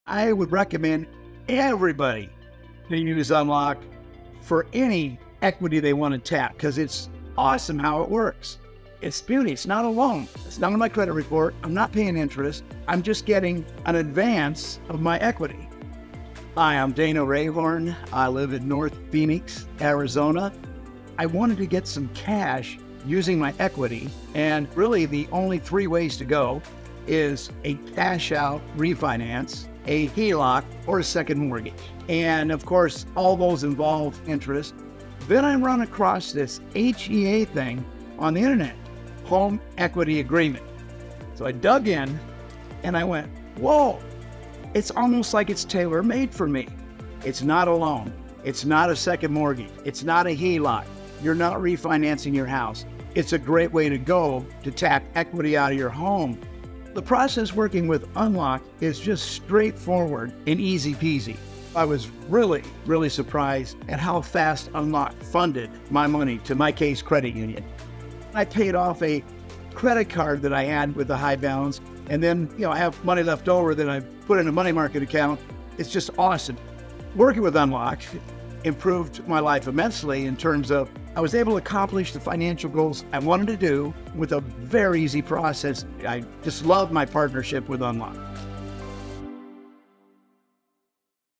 Unlock Technologies Success Story: Dana Rayhorn
Dana needed cash to pay off some credit card debt and some other bills. He looked into HELOCs, second mortgages, even cash-out refi. Then he found us. Here's Dana, in his own words, telling us how it all worked out.
Is a home equity agreement (HEA) right for you?